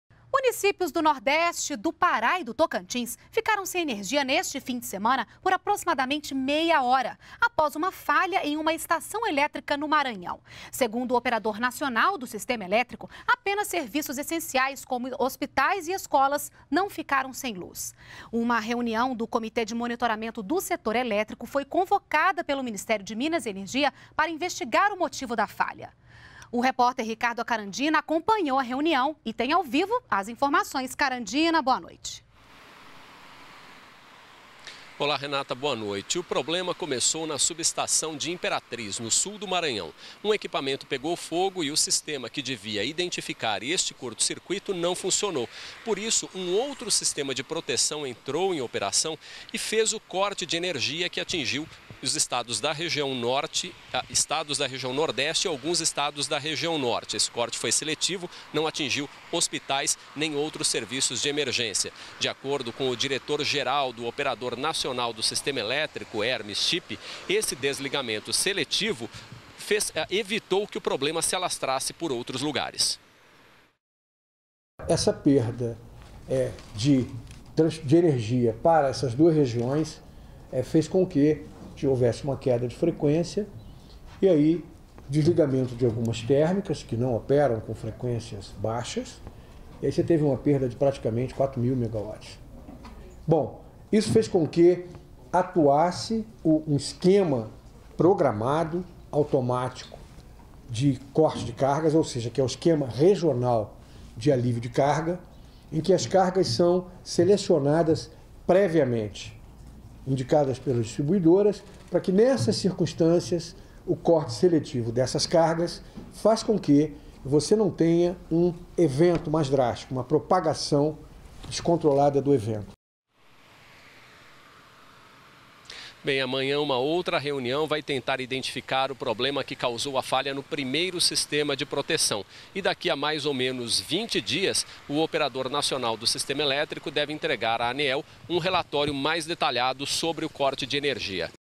Problema em subestação de energia no Maranhão será investigado
NBR NOTÍCIAS - 24.09.12: Cidades do Nordeste, Pará e Tocantins ficaram sem energia elétrica neste fim de semana por aproximadamente meia hora. A queda aconteceu devido a uma falha na subestação de Imperatriz no estado do Maranhão. Um  equipamento que deveria ter identificado o curto-circuito e desligado a energia localmente não funcionou. Outro aparelho conseguiu detectar o problema e cortou a energia automaticamente para evitar danos ao sistema. Segundo o Operador Nacional do Sistema Elétrico, apenas serviços essenciais, como hospitais e escolas não ficaram sem luz. O Comitê de Monitoramento do Setor Elétrico foi convocado pelo Ministério de Minas e Energia (MME) para investigar o motivo da falha.